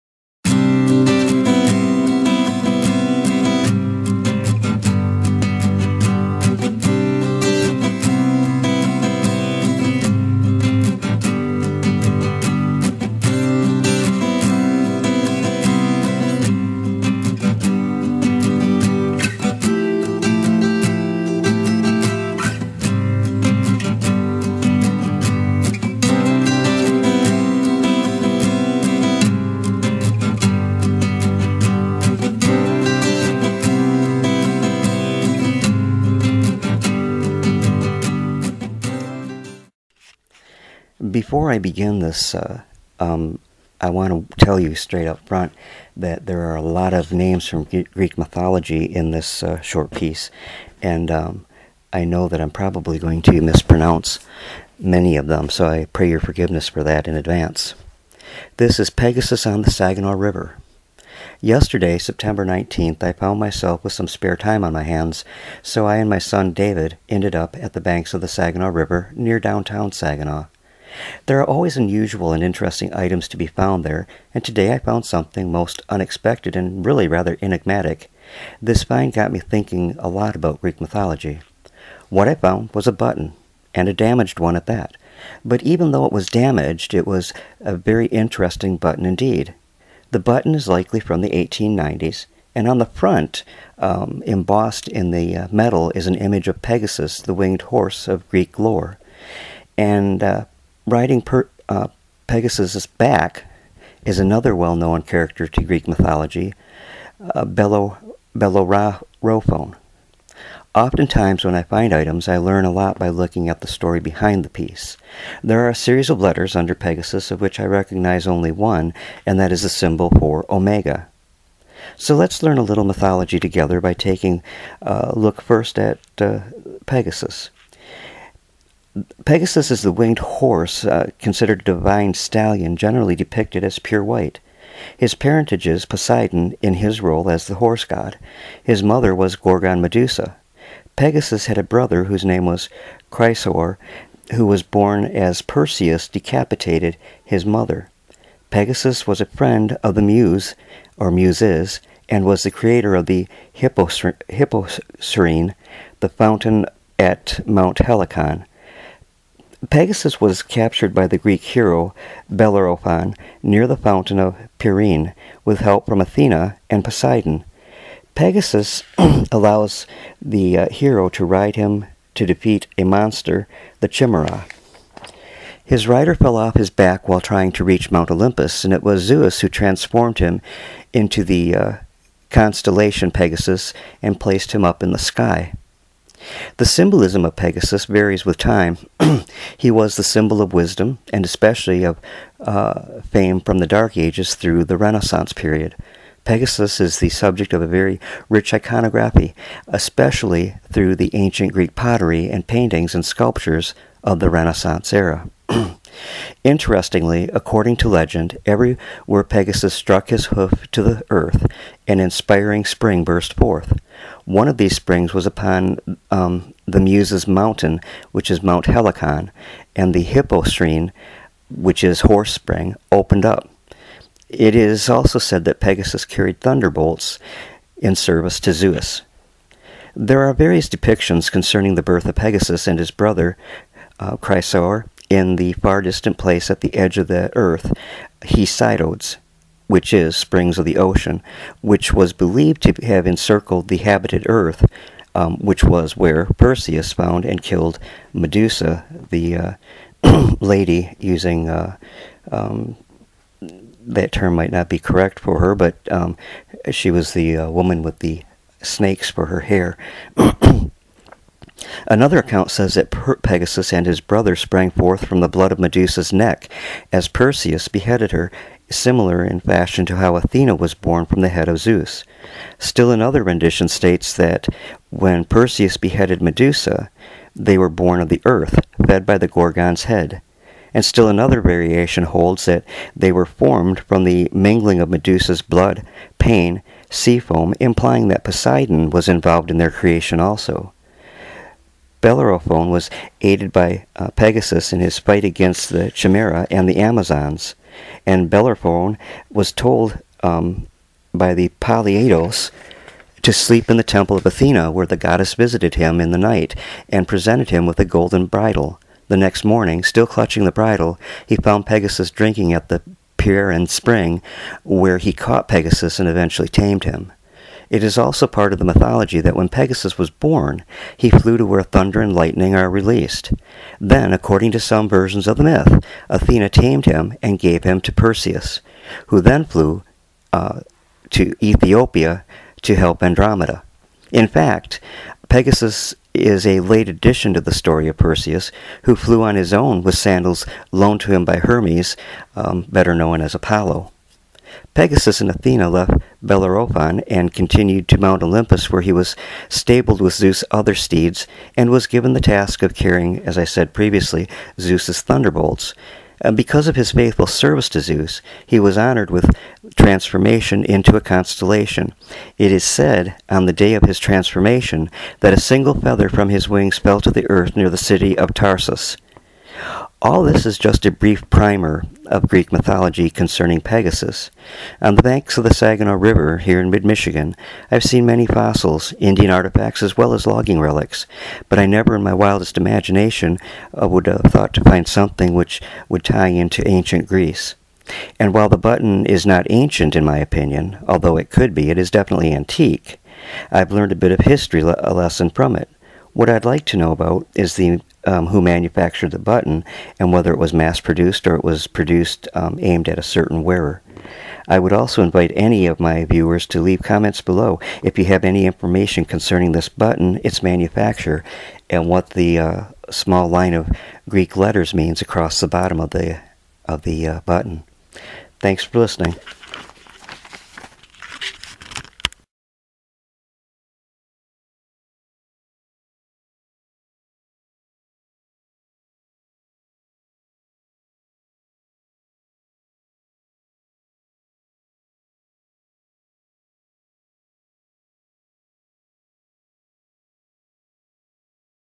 PEGASUS ON THE SAGINAW RIVER - ANCIENT GREECE REMEMBERED IN MID MICHIGAN
This short video is about an antique button I recently found on the Saginaw River.  The button is quite old, say 1880s to 1890s, but it calls to mind images of Ancient Greece and the colorful mythology of that culture.